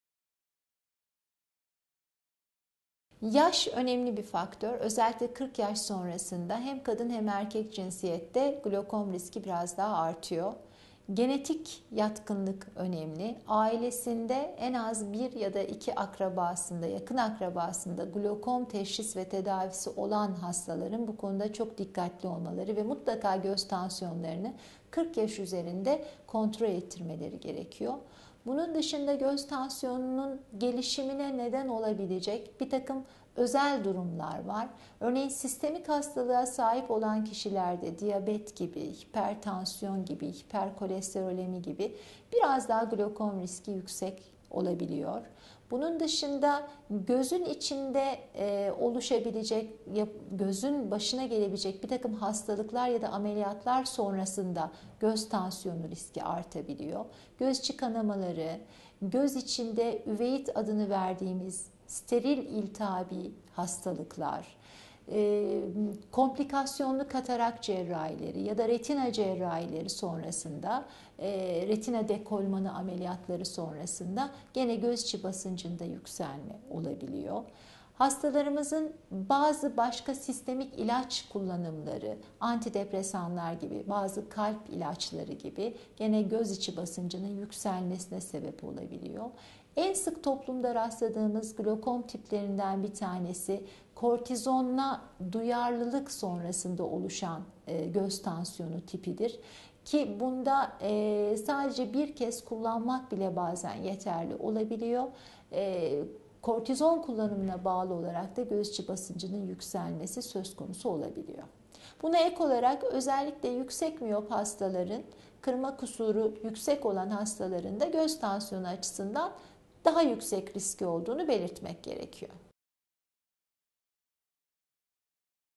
Glokom riskini artıran faktörler var mıdır?
Acıbadem Maslak Hastanesi, Göz Hastalıkları Uzmanı Dr. Gülbin Saltık, Göz Hastalıkları üzerine soruları yanıtlıyor.